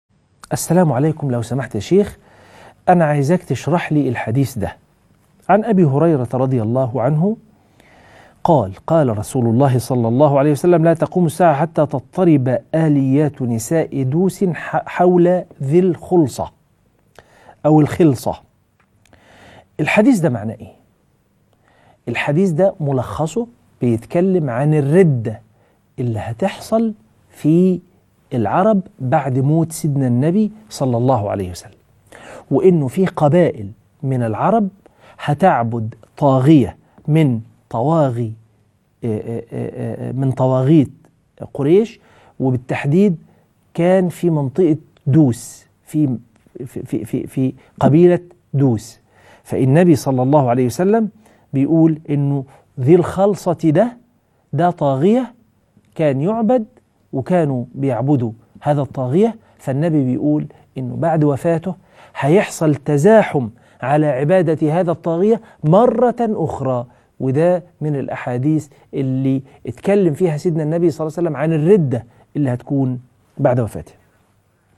فتاوى - الشيخ/ أحمد صبري - تفسير حديث " لا تقوم الساعة حتى تضطرب أليات نساء دوس ..."
فتاوى مع الشيخ أحمد صبري